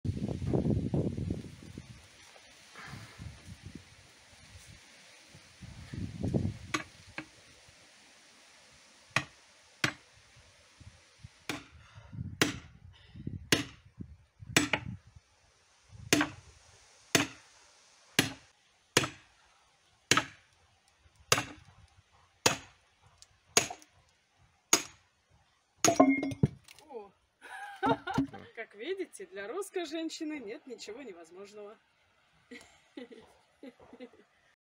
Девушка колет дрова
Лайфхак. Как расколоть толстое сучковатое бревно. (Краткая версия)